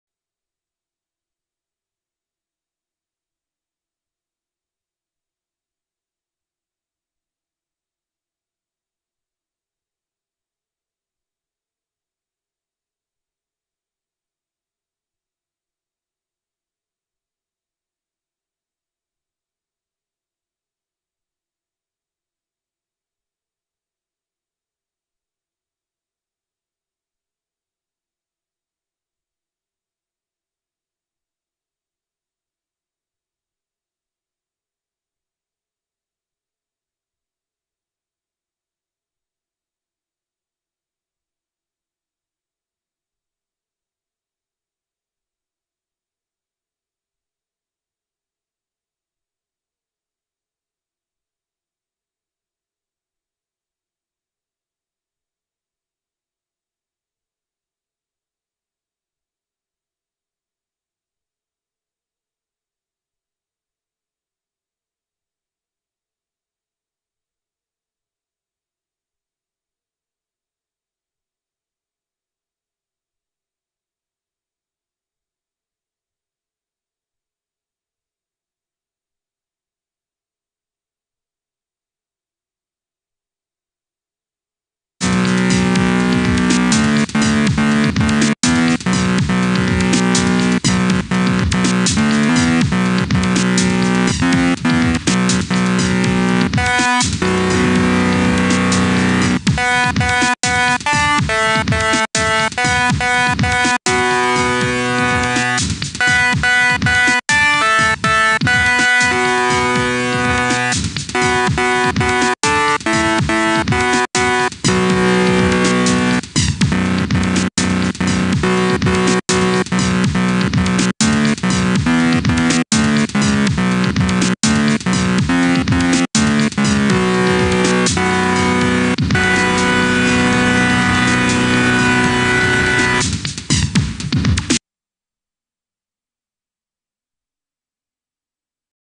Dubstep song (with horrible drums i have no rhyme sorry)
Just a quick dubstep song I made on lmms, and If you like my back ground here it is just right click and hit save Long Ass code I know